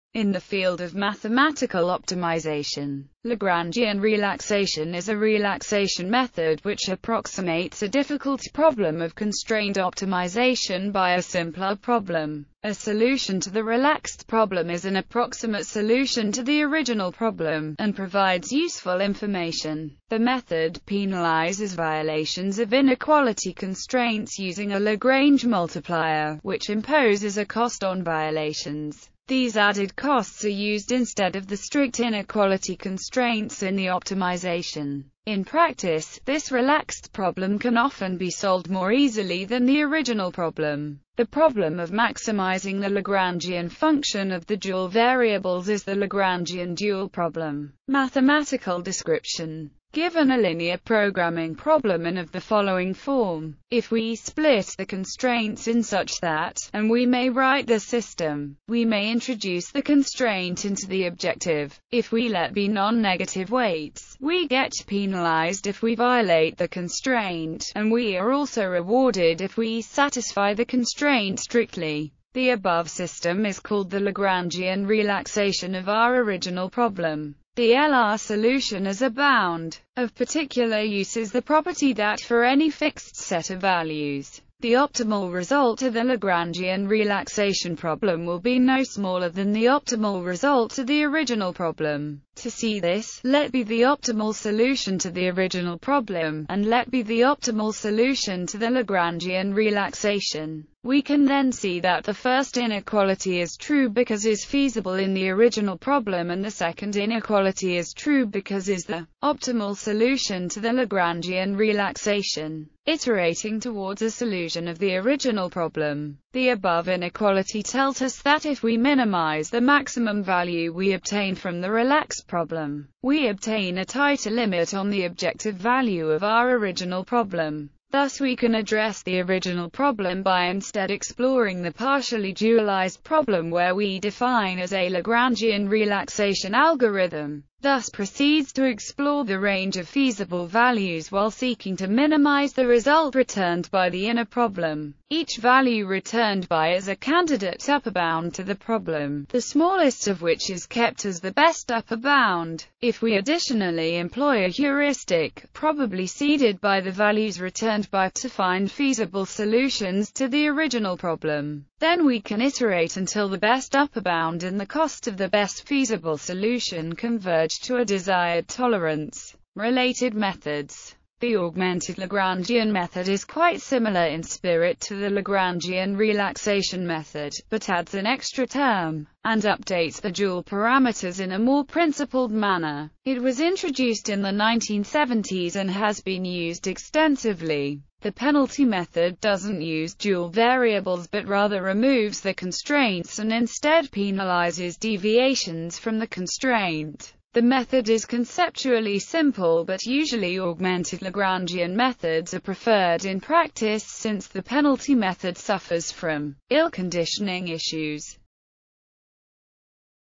Lagrangian relaxation
In the field of mathematical optimization, Lagrangian relaxation is a relaxation method which approximates a difficult problem of constrained optimization by a simpler problem.A solution to the relaxed problem is an approximate solution to the original problem, and provides useful information.